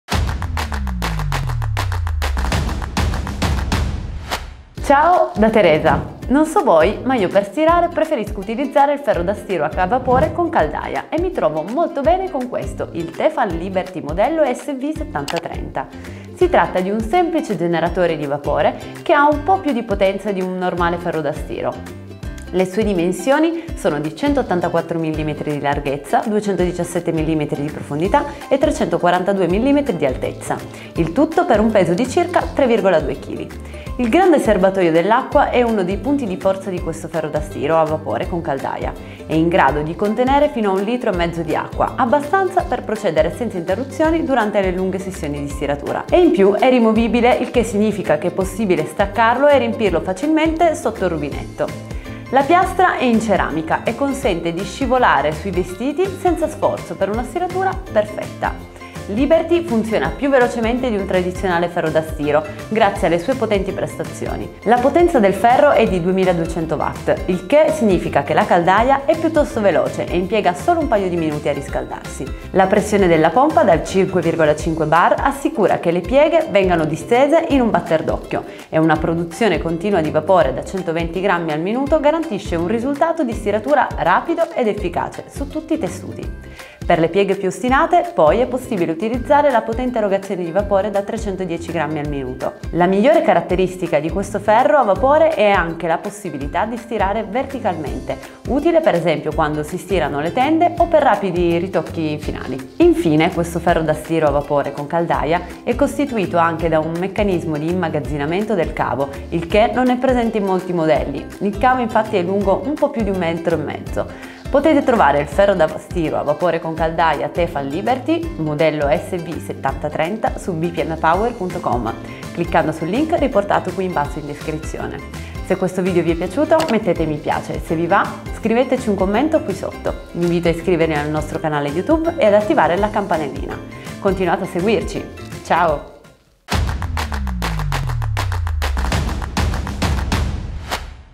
Tefal Liberty SV7030, ferro da stiro a caldaia economico e versatile
Parliamo un po' del Tefal Liberty modello sv7030. Si tratta di un semplice generatore di vapore che ha un po' più di potenza di un normale ferro da stiro. Le sue dimensioni sono di 184 mm di larghezza, 217 mm di profondità e 342 mm di altezza. Scopritelo con noi!